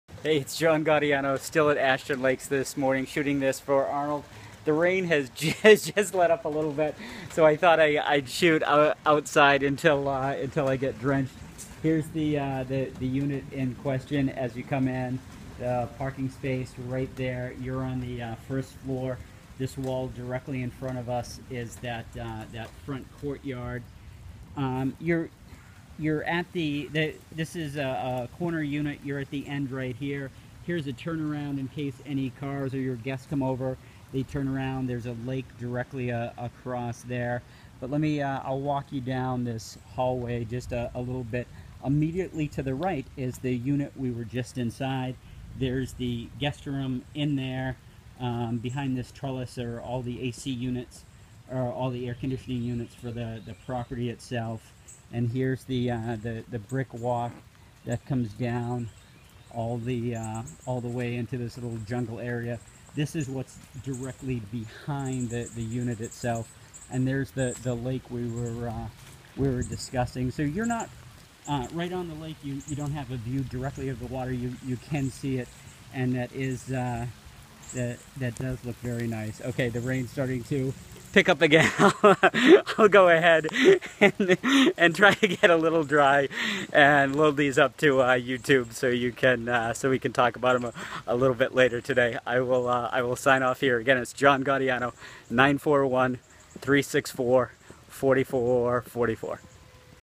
Private Client Tour | Ashton Lakes | Siesta Key Beach | 34231
Short tour of unit in Ashton Lakes near Siesta Key Beach.